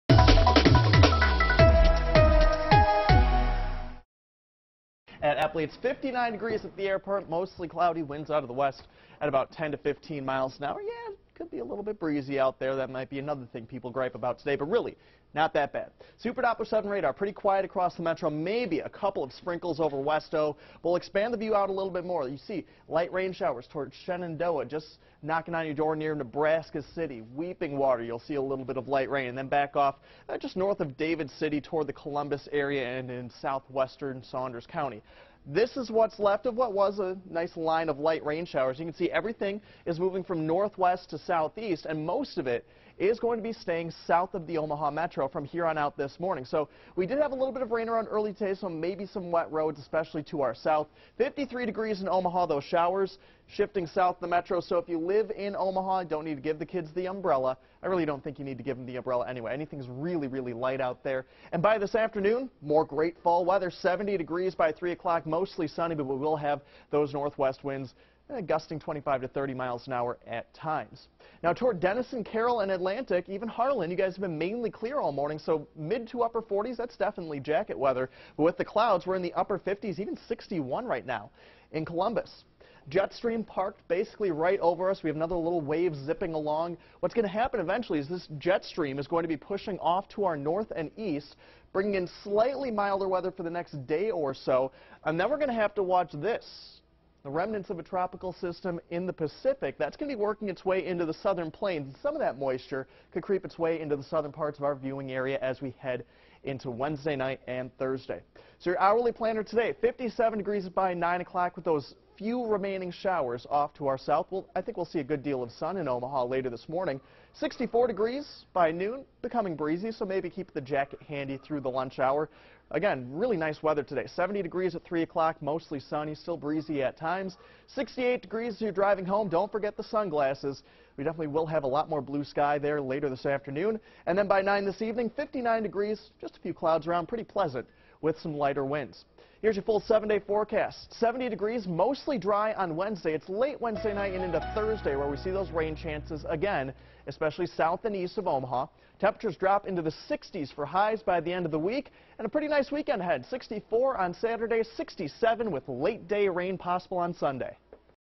Matt's Full Tuesday Forecast
Light rain shifting south this morning, followed by more sun and breezy conditions. Meteorologist Matt Serwe has your full Weather Now forecast.